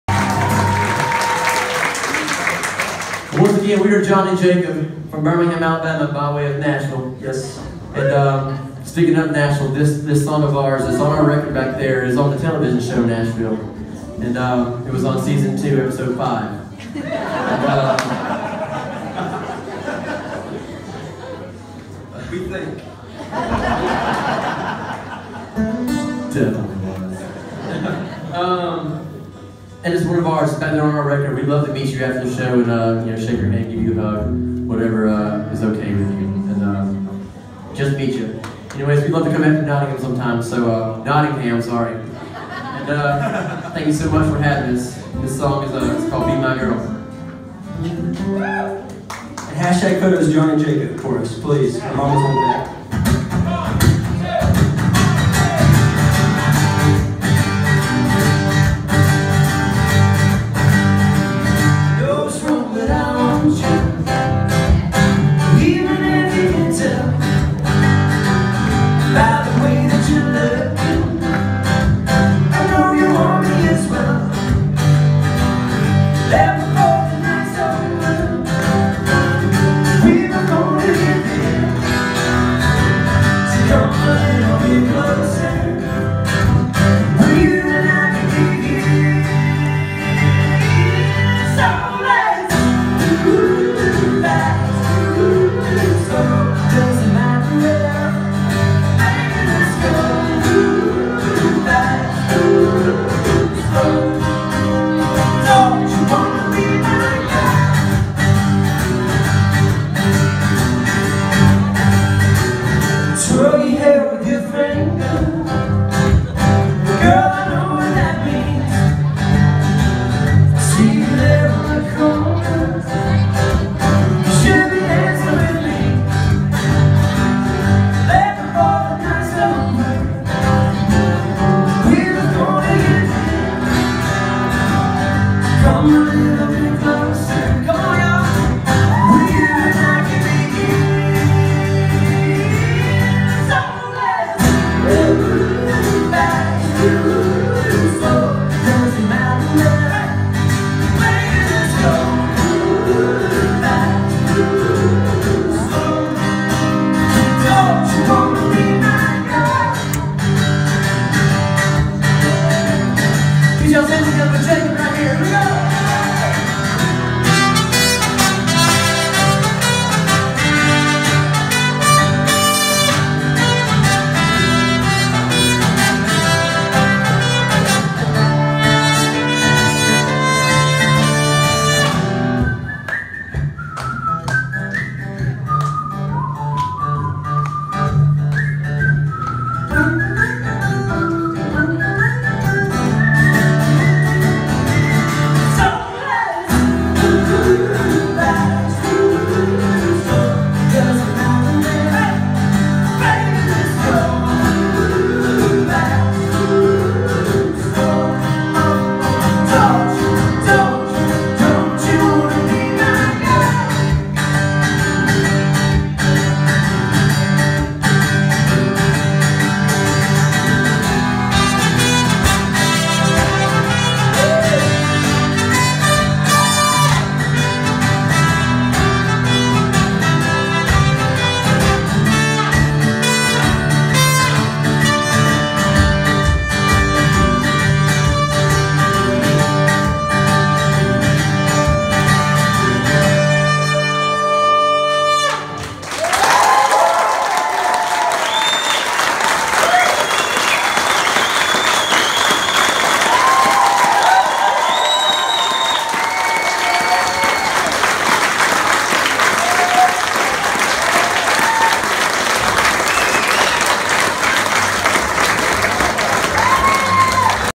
John & Jacob LIVE @ Rescue Rooms Nottingham! 10th Nov 2015
John and Jacob live in Nottingham, U.K. 10th November 2015. AMAZING!